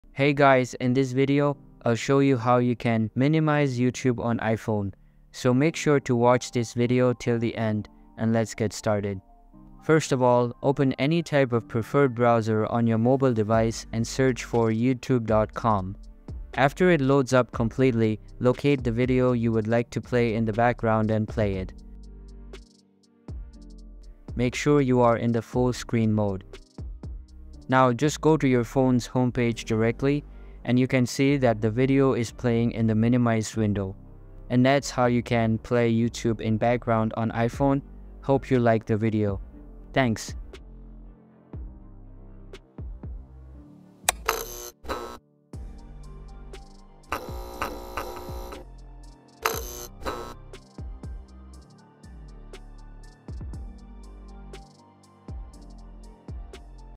How to Minimize Youtube On Iphone - Step By Step (2024)
How to Minimize Youtube On Iphone

In this video I'll show you how to Minimize Youtube On Iphone. The method is very simple and clearly described in the video. Follow all of the steps & Minimize Youtube On Iphone